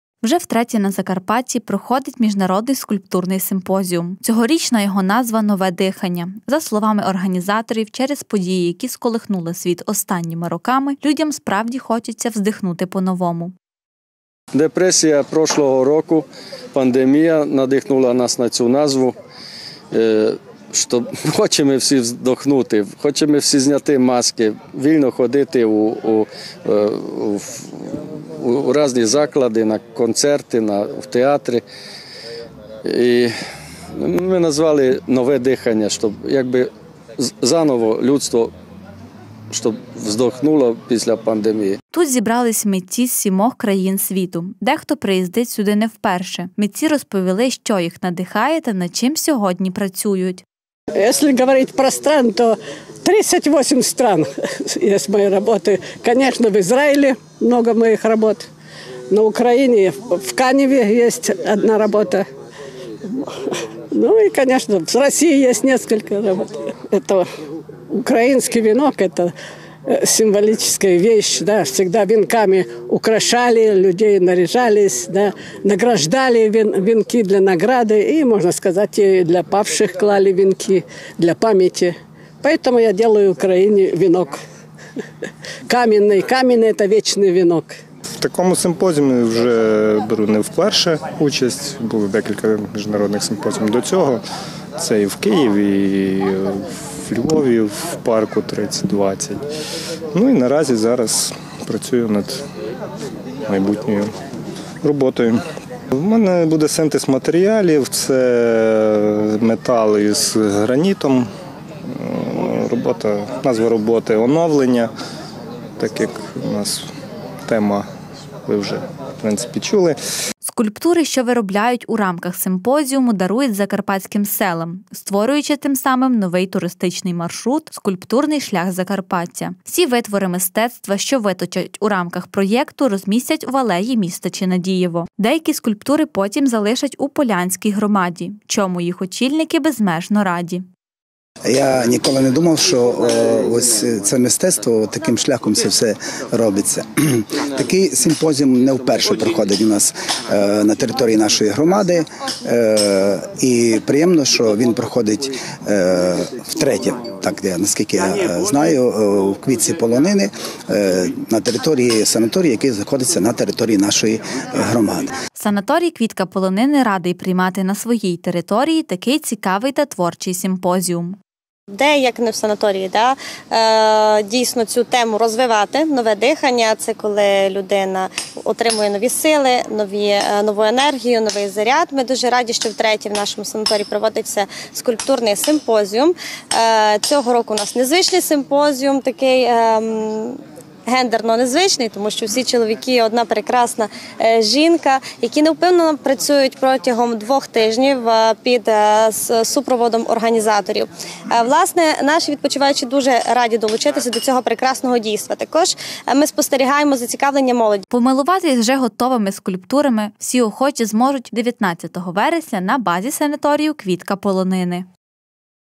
«Нове дихання». Міжнародний симпозіум
На базі санаторію «Квітка Полонини» відбувається 4 карпатський міжнародний скульптурний симпозіум. Під назвою «Нове дихання». В якому беруть участь  7 скульпторів з різних країн світу. Вже втретє в Закарпатті проходить Міжнародний скульптурний симпозіум. Цьогорічна його назва «Нове дихання». За словами організаторів, через події, які сколихнули світ останніми роками людям справді хочеться чогось вдихнути по-новому. Тут зібралися митці з 7 країн світу. Дехто приїздить не вперше. Митці розповіли, що  їх надихає та над чим  сьогодні працюють. Скульптури, що роблять у рамках симпозіуму дарують закарпатським селам, створюючи тим самим нові туристичний маршрут «Скульптурний шлях Закарпаття» . Всі витвори мистецтва, що виточать у рамках проєкту розмістять в алеї міста Чинадієво.Деякі скульптури потім залишать у Полянській громаді. Чому її очільники безмежно цьому  раді. Санаторій «Квітка Полонини» радий приймати на своїй території такий цікавий та творчий симпозіум. Помилуватись вже готовими скульптури всі охочі зможуть вже 19 вересня на базі санаторію «Квітка Полонини».